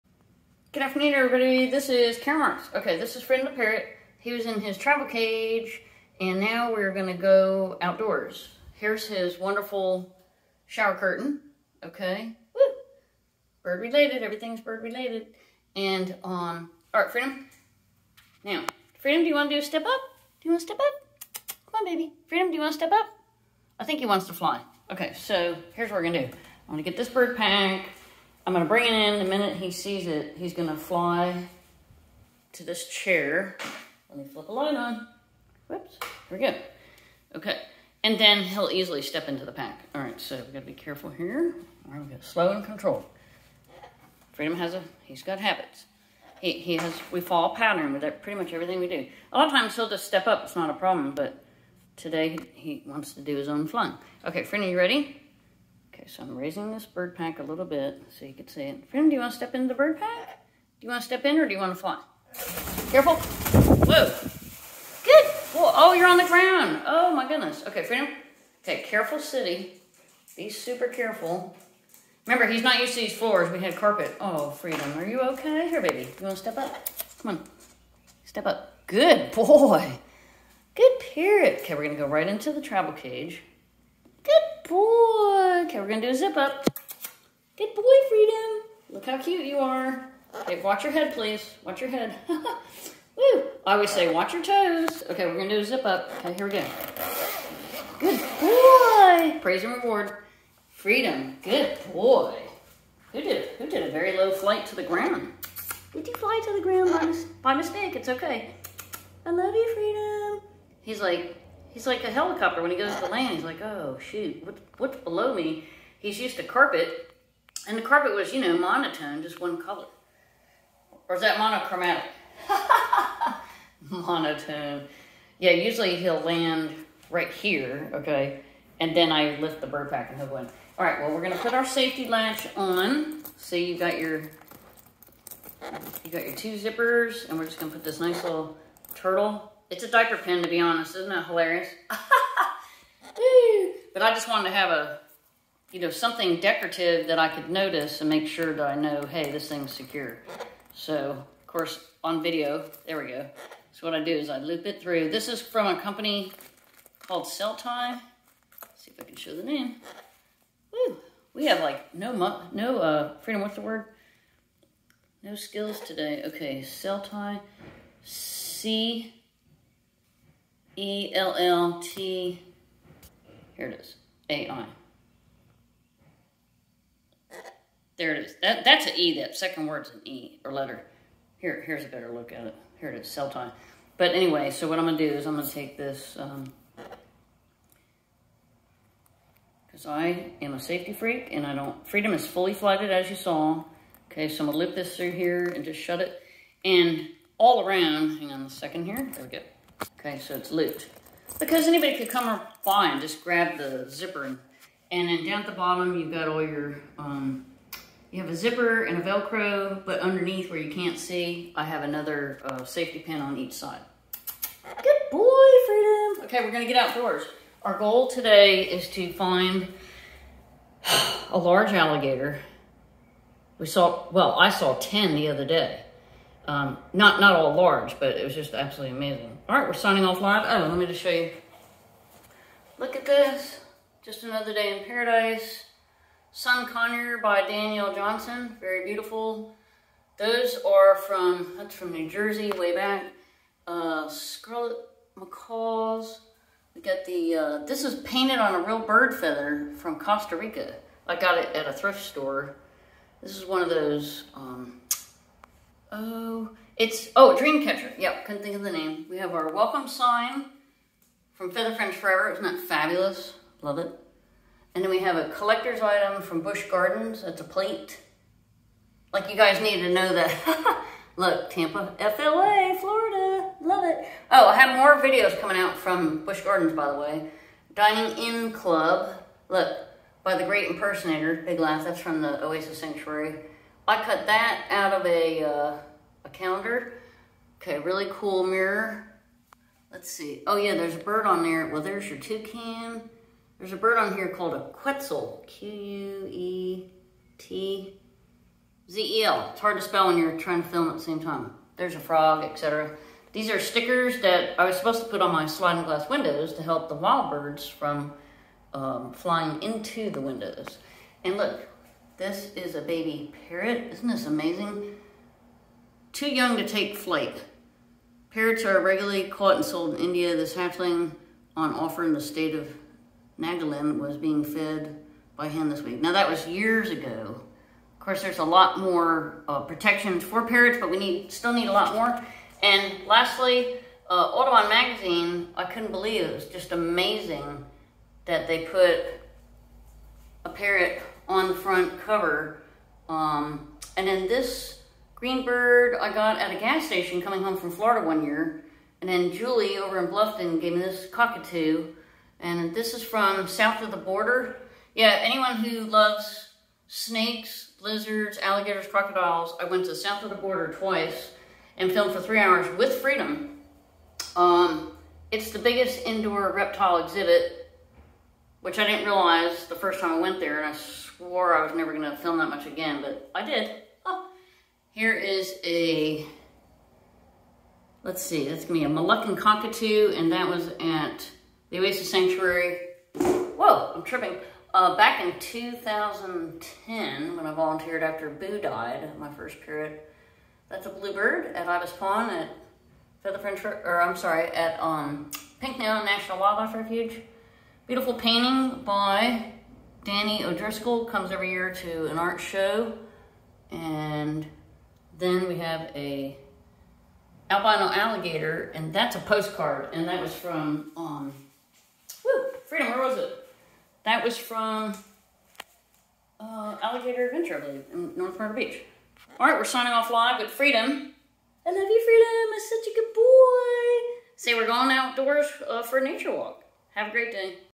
Freedom the Parrot Flies from Bathroom to Land on Floor & Steps Up to Bird Pak! Hilton Head Island
Freedom the Parrot Flies from Bathroom to Land on Floor & Steps Up to Bird Pak! Hilton Head Island, SC